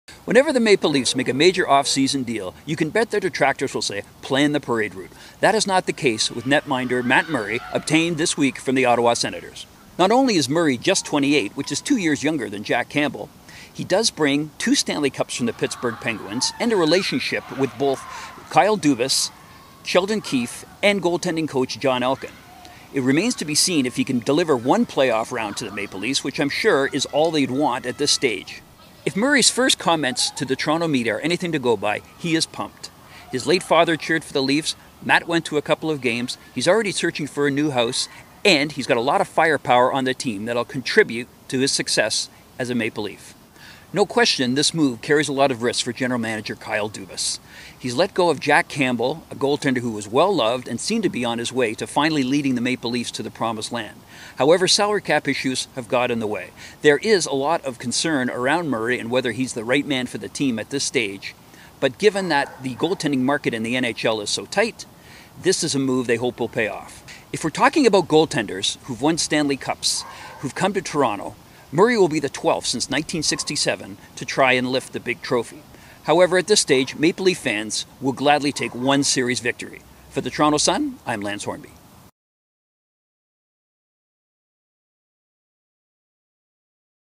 LEAFS NATION: Toronto acquires goalie Matt Murray from Ottawa
Toronto Sun's Lance Hornby on the Toronto Maple Leafs newest acquisition, goalie Matt Murray from the Ottawa Senators.